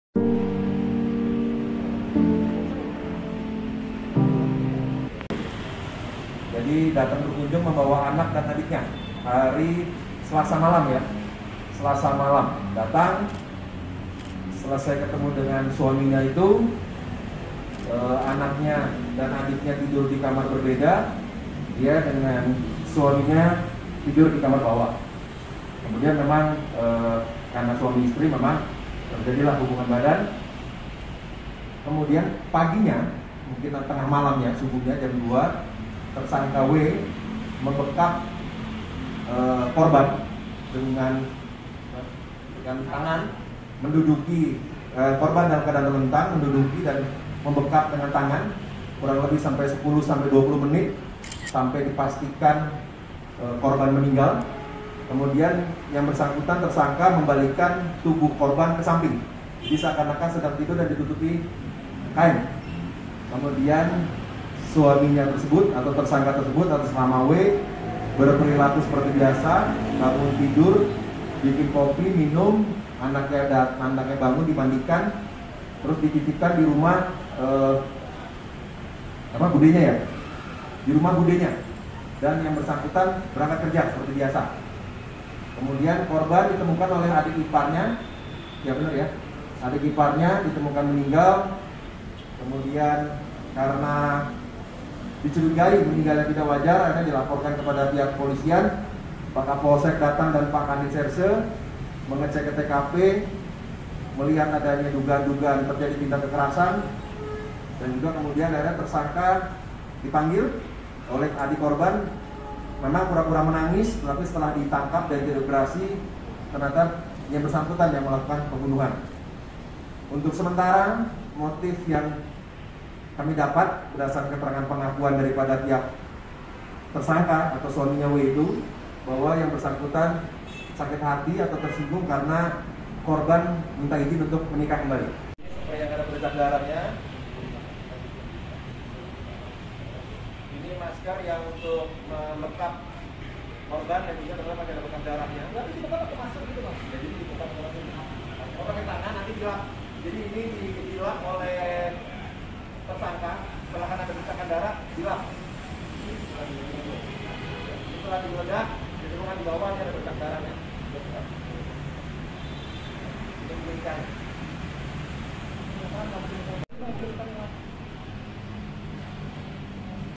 Kronologis Suami 8unuh Istri di Duren Sawit, K0rban Dibekap Usai Berhubungan Intim
WARTAKOTALIVE.COM, JATINEGARA -- Kronologis W (41), suami yang tega membunuh istrinya, SS (29) di kontrakannya di Jalan Pondok Kelapa Selatan VI RT 09/RW 05, Pondok Kelapa, Duren Sawit, Jakarta Timur mulai terkuak.

Kapolres Metro Jakarta Timur Kombes Pol Budi Sartono mengatakan awalnya korban datang dari kampung untuk menemui suaminya bersama anak dan adiknya, Selasa (18/1/2022) malam.

Korban dan pelaku yang sudah lama tak bertemu sempat berhubungan intim untuk melepas rindu. Namun pada Rabu (19/1/2022) dini hari pelaku membunuh korban dengan cara membekapnya.

“Tersangka W lalu membekap korban dengan tangan kurang lebih hingga 10-20 menit sampai dipastikan korban meninggal,” ucap Budi, Jumat (21/1/2022).

Pelaku kemudian membalikkan tubuh korban ke samping seakan-akan sedang tidur dan ditutupi kain. Setelahnya W berperilaku biasa dan pergi ke salon tempatnya bekerja sebagai hair stylish.

“Kemudian korban ditemukan oleh adik iparnya ditemukan meninggal, kemudian karena dicurigai meninggalnya tidak wajar, dilaporkan ke polisi,” kata Budi.